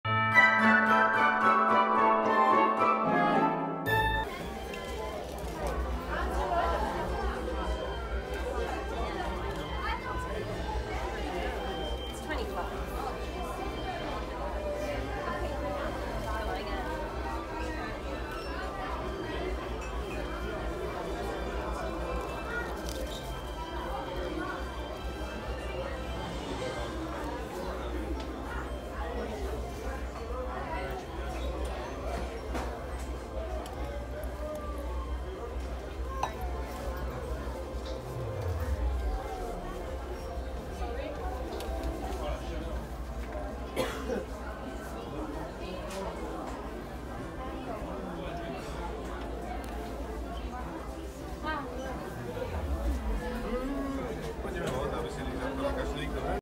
Boutique Constellations - Nouveautés ornements sapin et décorations - Disneyland Paris
Boutique : Constellations
Parc :  Disneyland Paris
Notre tik tok : AddictParksFR